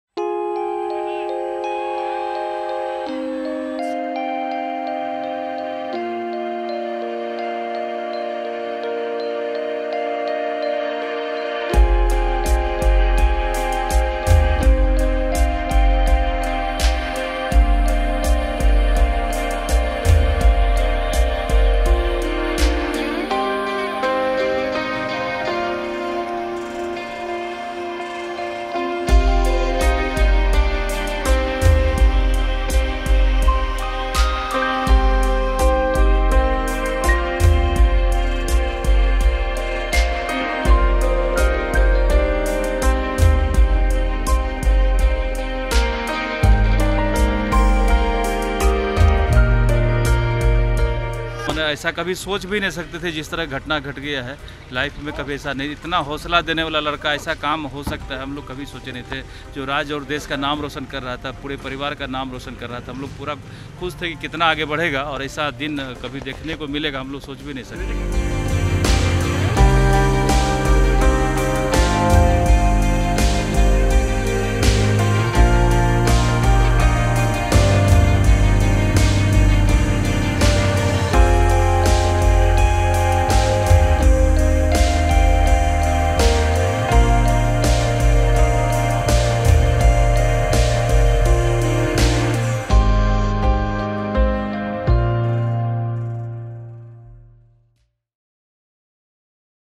Bollywood actor Sushant Singh Rajput cremated in Mumbai
The last rites for 34-year-old Bollywood actor, Sushant Singh Rajput, was held in India's western city of Mumbai on Monday (June 15).

Rajput, who won acclaim for his role in the 2016 biopic of then Indian cricket captain Mahendra Singh Dhoni, was found dead at his home on Sunday (June 14) and an investigation is ongoing, police said, declining to give further details.